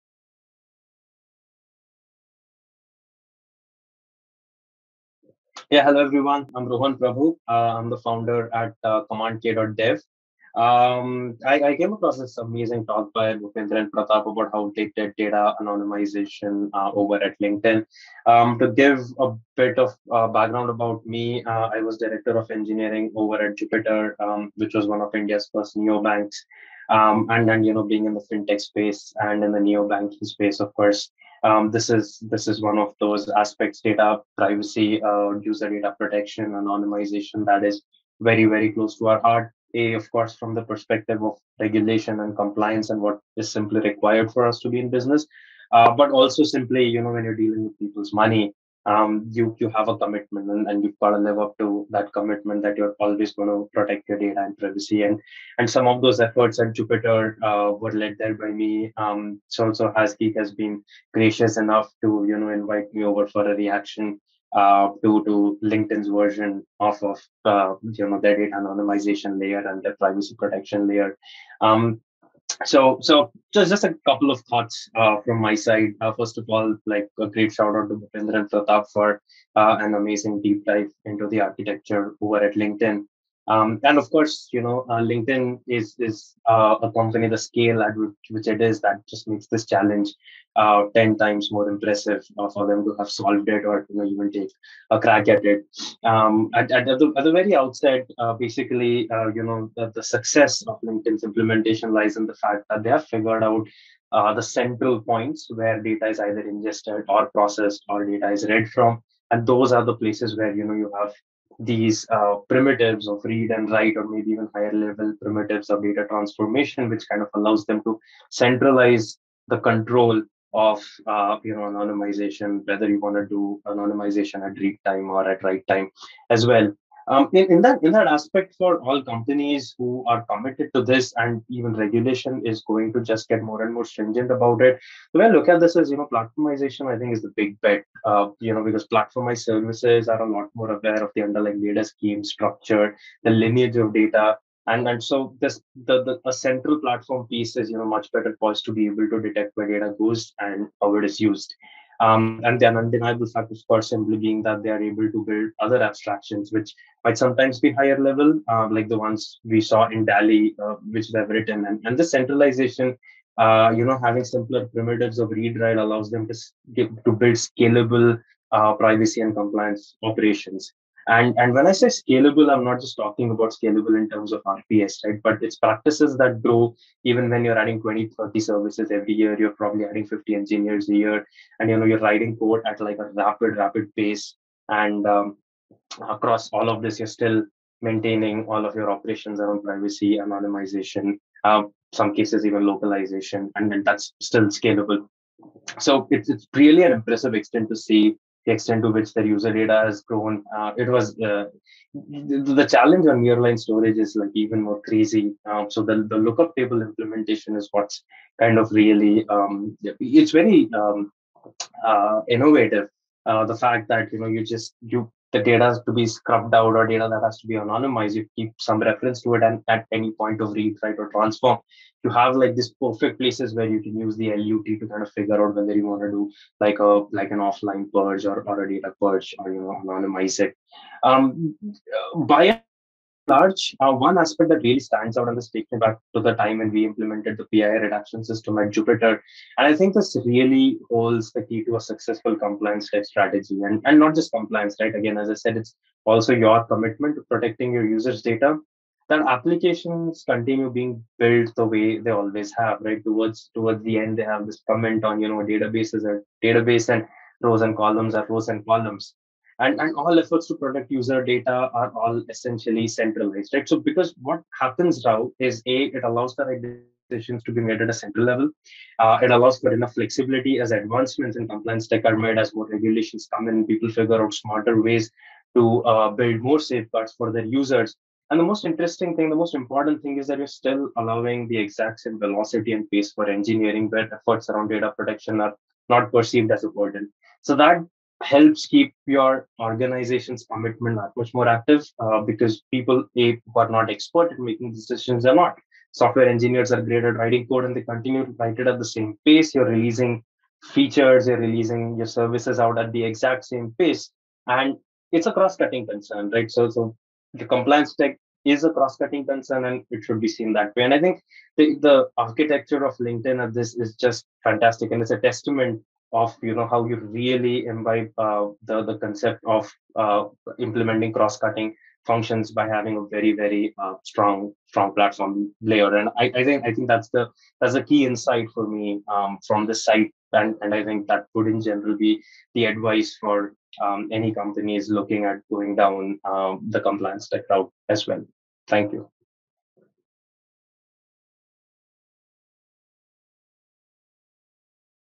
User Data Privacy Architecture at LinkedIn
Speaker: Rohan Prabhu, Director of Engineering at Jupiter